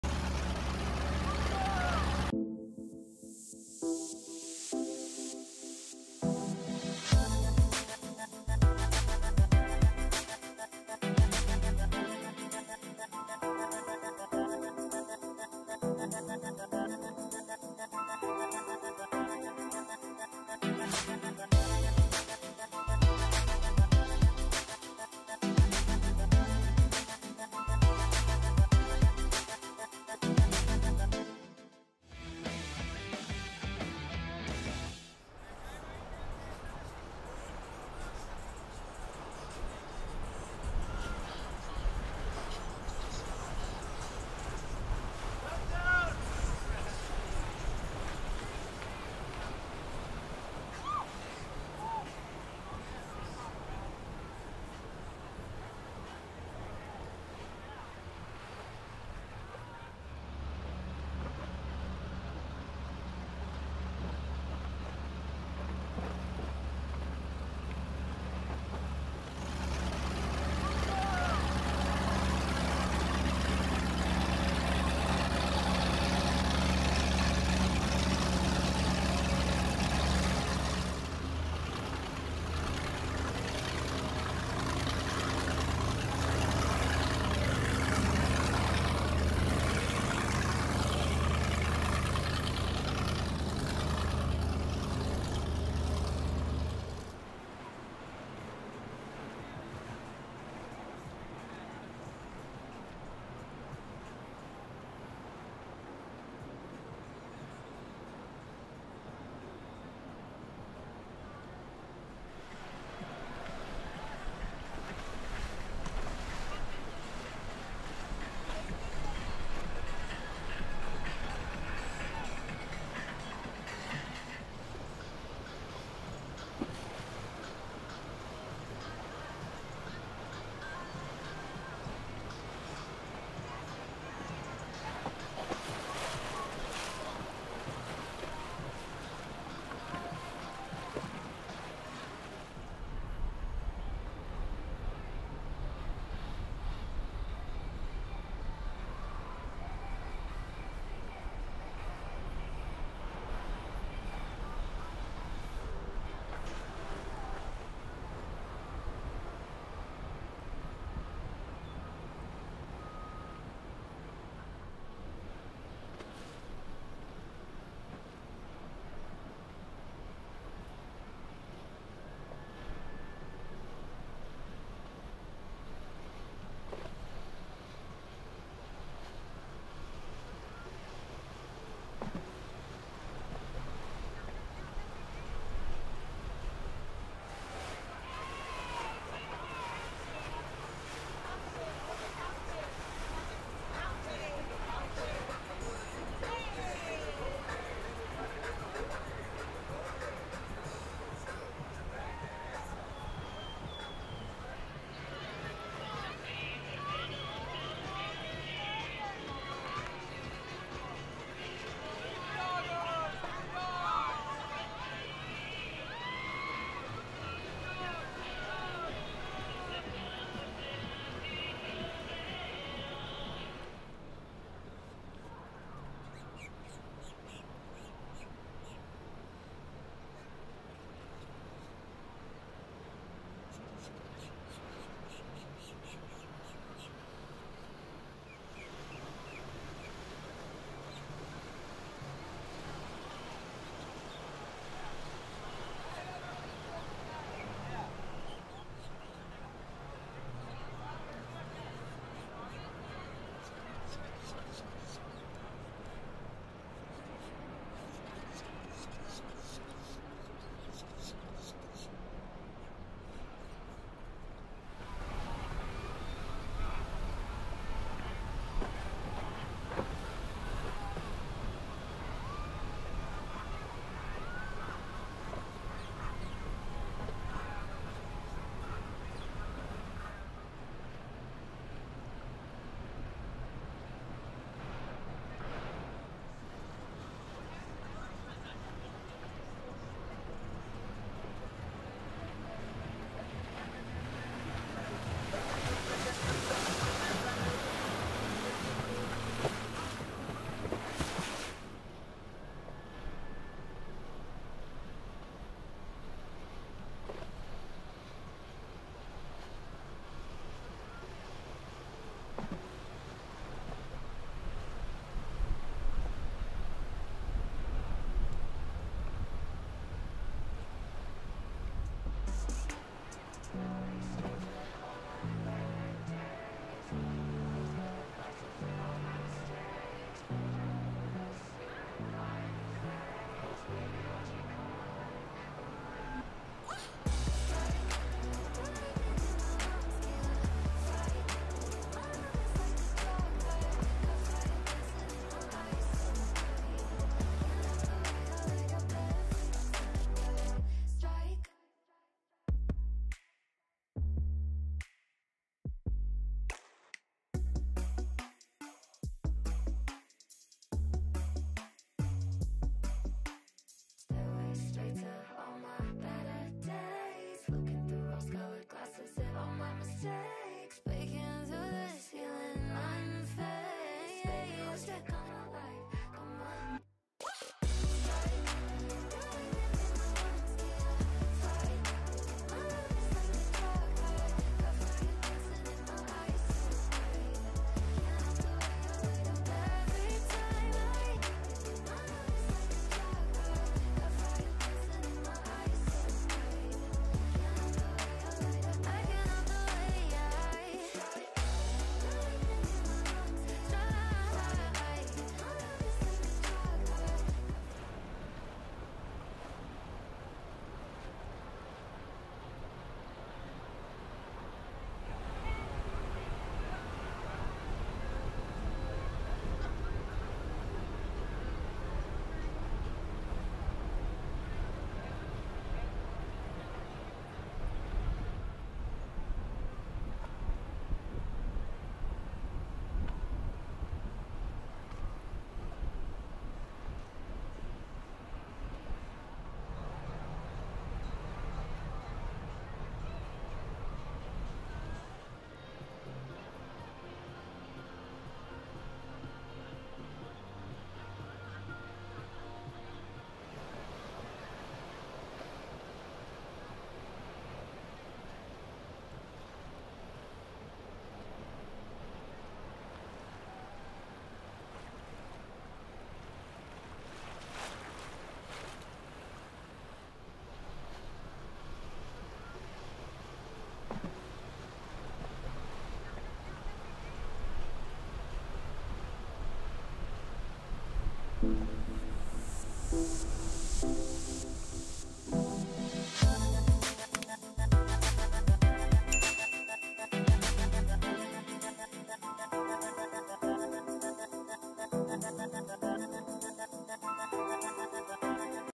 WHAT IS GOING ON IN MIAMI? FALL SEASON | BOAT ZONE
Haulover and the Miami River are incredible places! When it comes to an epic party in Miami, there's no place like the Miami River with music, boats, drinks, girls and much more.   Buy a Super Thanks, which directly supports Boat Zone.
Interested in advertise your business with us? Send us your questions or visit our